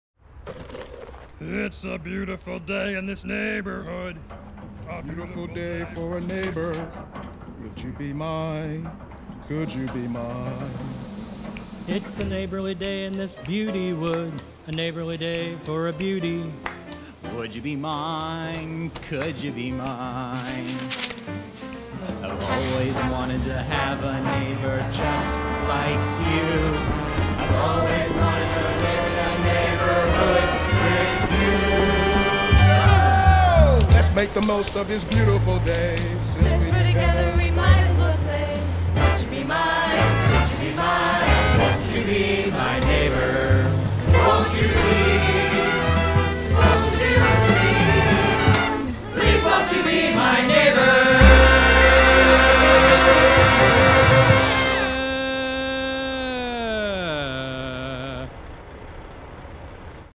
A Beautiful Day — Ozinga 90th Anniversary
As a fourth-generation family-owned American business, we have a lot to be thankful for, but most of all, we're thankful to have been your neighbor for the last 90 years.

It's not often a family business lasts this long and we know it's because of our people—our customers, our coworkers and our communities, past and present. We're greatly humbled and looking forward to the next 90 years.